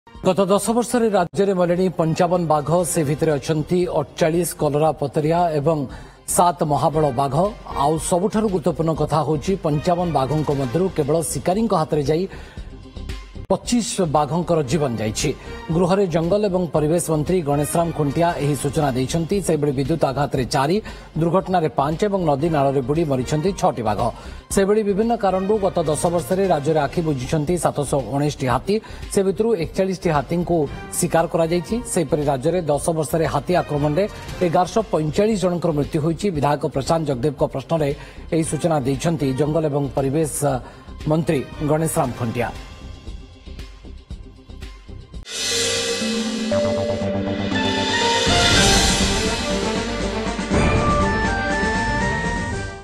୧୦ ବର୍ଷରେ ୪୮ କଲରାପତରିଆ, ୭ ମହାବଳ ବାଘଙ୍କ ମୃତ୍ୟୁ
ଗତ ୧୦ ବର୍ଷରେ ରାଜ୍ୟରେ ମଲେଣି ୫୫ ବାଘ । ସେ ଭିତରେ ଅଛନ୍ତି ୪୮ କଲରାପତରିଆ ଓ ୭ ମହାବଳ ବାଘ। ଆଉ ସବୁଠୁ ଗୁରୁତ୍ବପୂର୍ଣ୍ଣ କଥା ହେଲା, ୫୫ ବାଘଙ୍କ ମଧ୍ୟରୁ କେବଳ ଶିକାରୀଙ୍କ ହାତରେ ଯାଇ ୨୫ ବାଘଙ୍କ ଜୀବନ ।